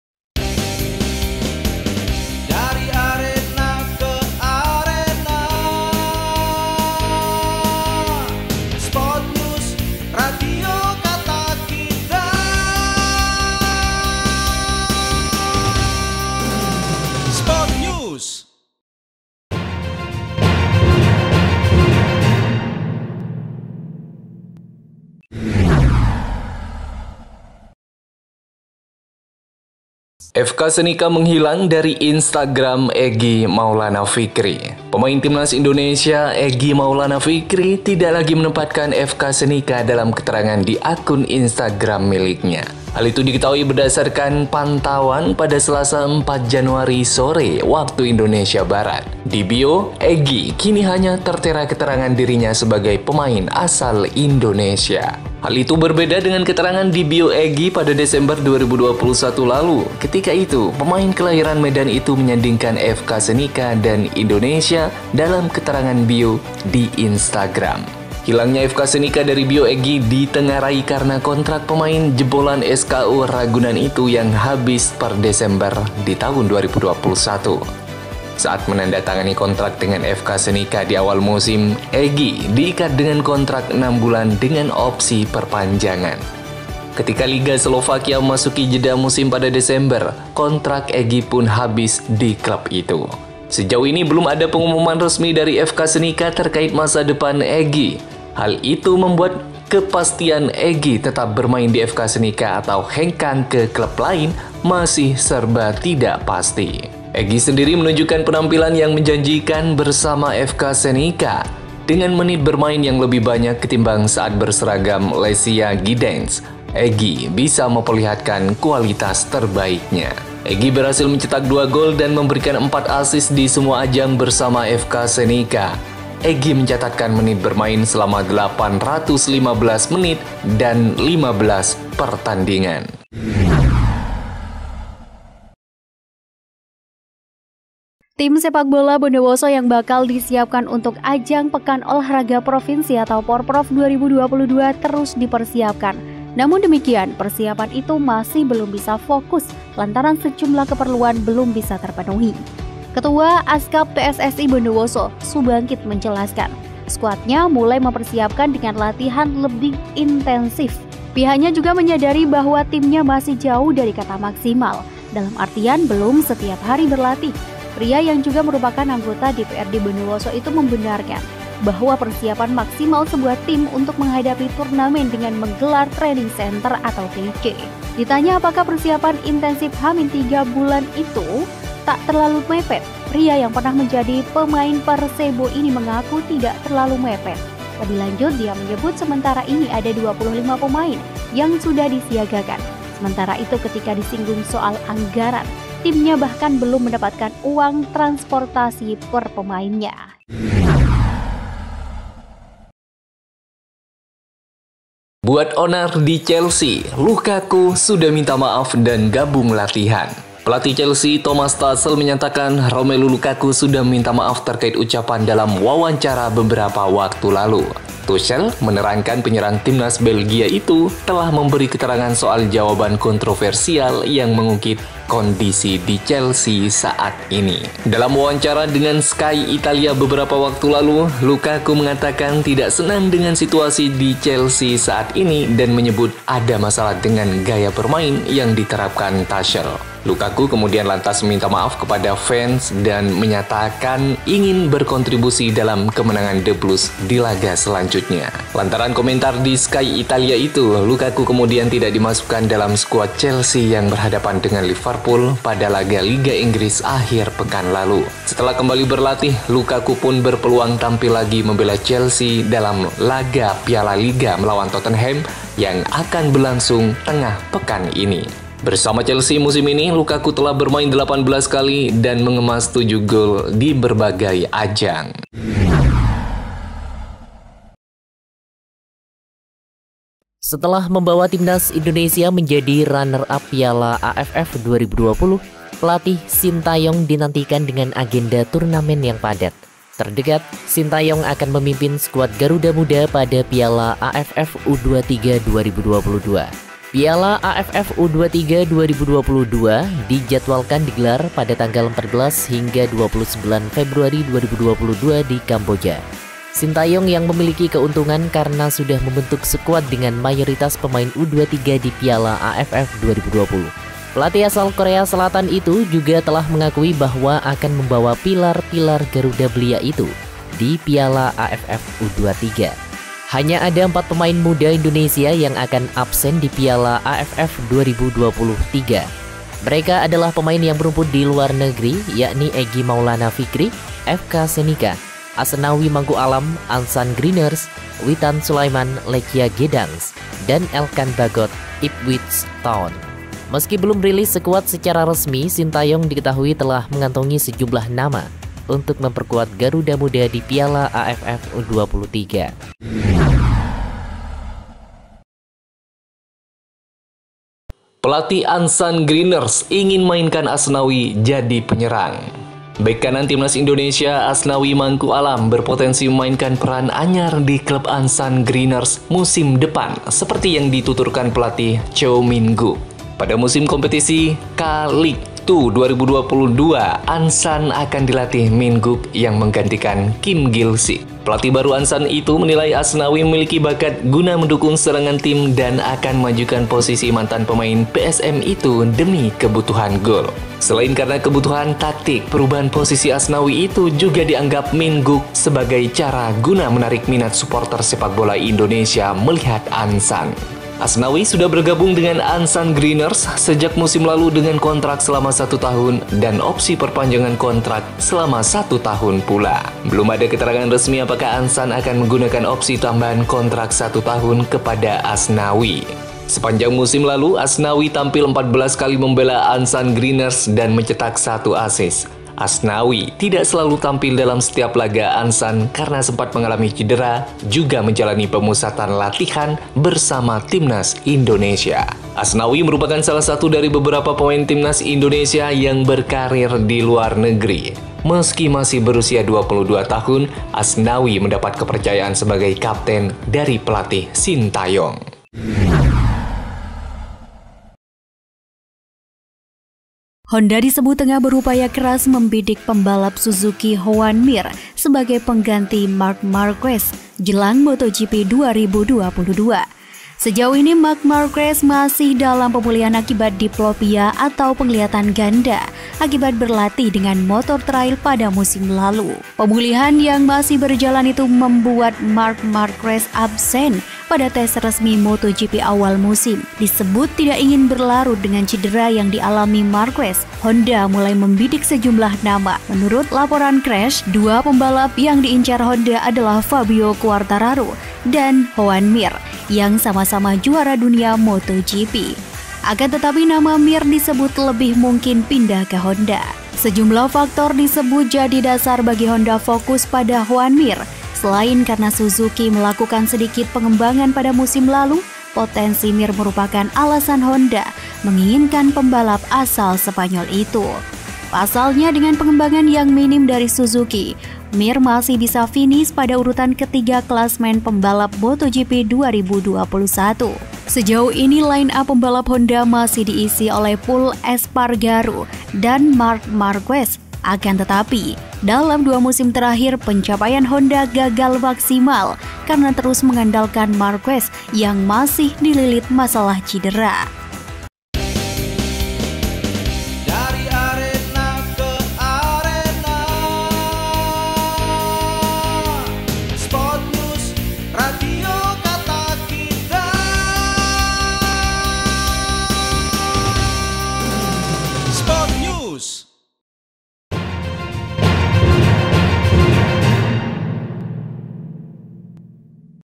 ASNAWI MULAI TUNJUKKAN SKILL AUTO JADI PEMAIN PENYERANG - SPORT NEWS
Radio Kata Kita adalah Channel yag mengulas berita terpercaya dan terbaru hari ini. konteks berita yang berkualitas  dan informatif.